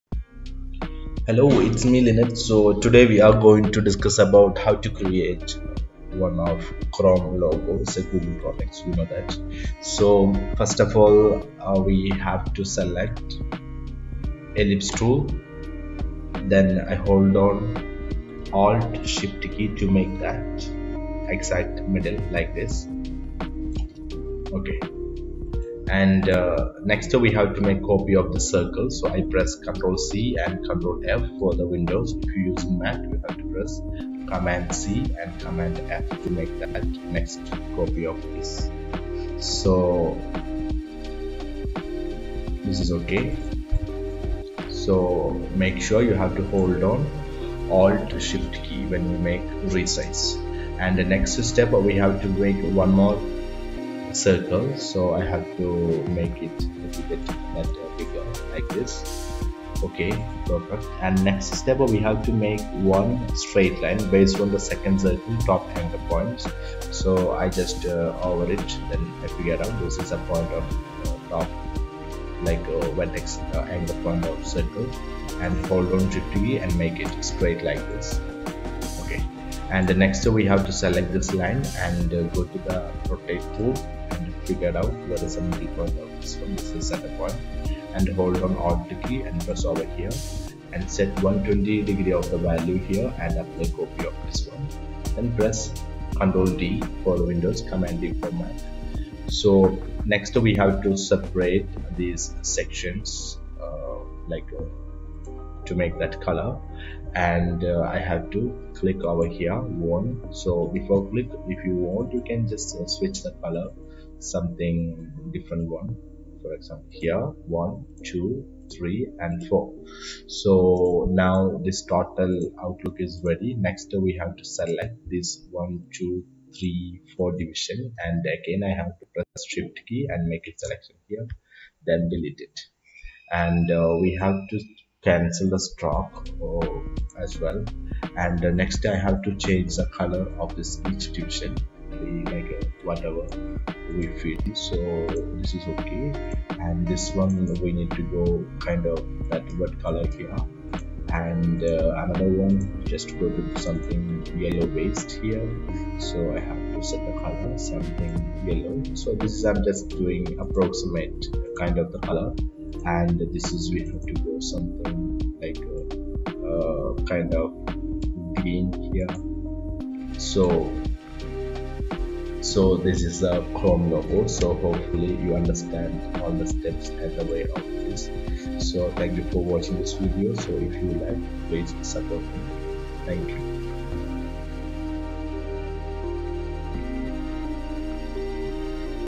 google chrome logo vector in illustrator tutorial-adobeillustrator
Here i upload a google chrome logo for beginners. i hope it will help you to get idea, please try this everyone..Thanks